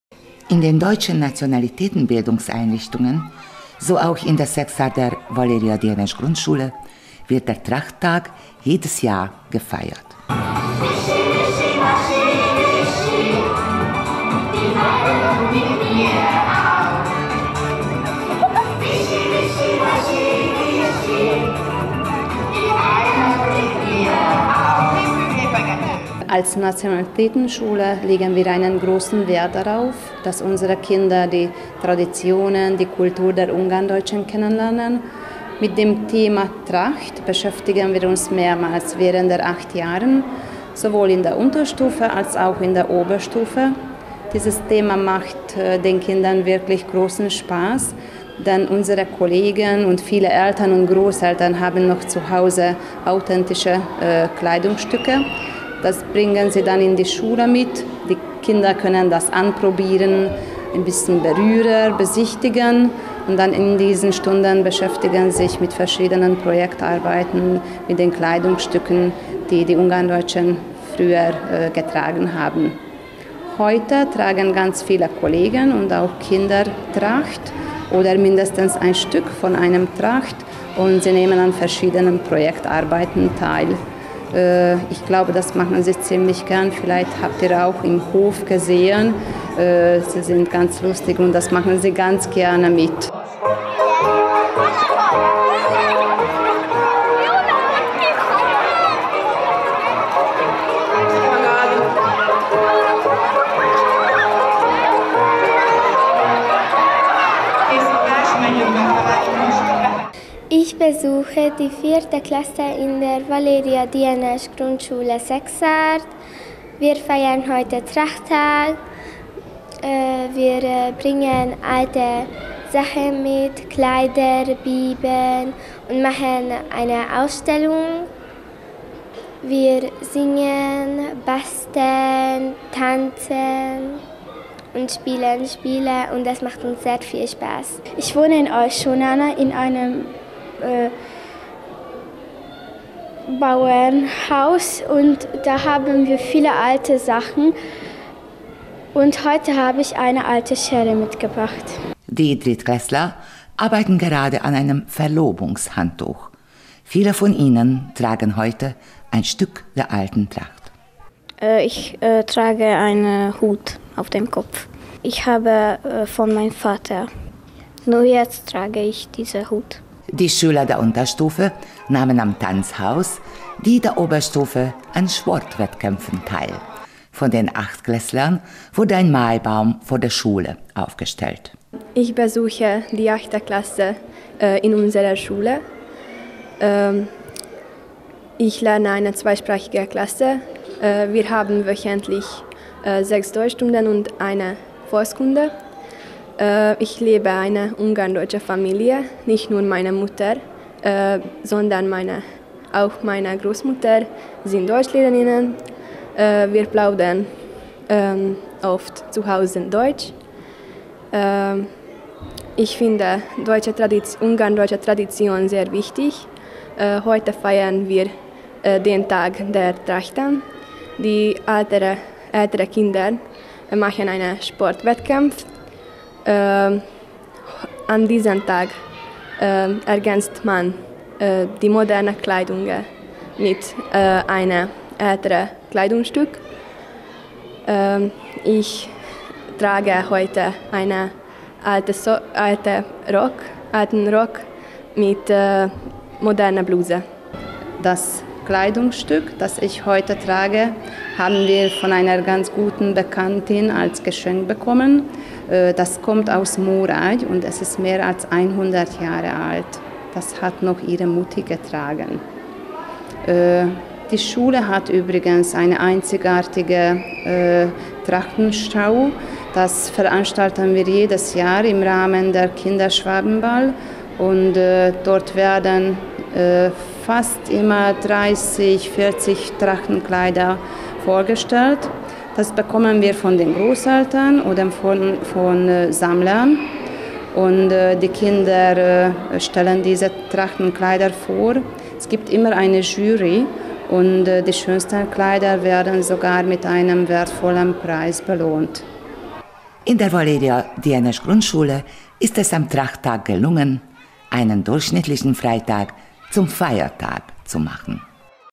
A német népviselet napja / TrachtTag
A Magyarországi Német Kulturális és Információs Központ és Könyvtár immár nyolcadik alkalommal hirdette meg a magyarországi német népviselet napját. Idén április 29-én öltötték fel óvodások, iskolások, csoportok, családok országszerte a német népviselet egy darabját, hogy a mindennapokban is bemutassák őseink örökségét. A szekszárdi Dienes Valéria Általános Iskolában színes programokkal ünnepelték a német viselet napját.

 Das Ungarndeutsche Kultur- und Informationszentrum und Bibliothek verkündete bereits zum achten Mal den Tag der ungarndeutschen Tracht. Heuer zogen am 29. April Kindergarten- und Schulkinder, Gruppen und Familien landesweit ein Stück der ungarndeutschen Tracht an, um auch im Alltag das Erbe unserer Ahnen zu zeigen. In der Seksarder Valeria Dienes Grundschule wurde der Trachttag mit bunten Programmen gefeiert.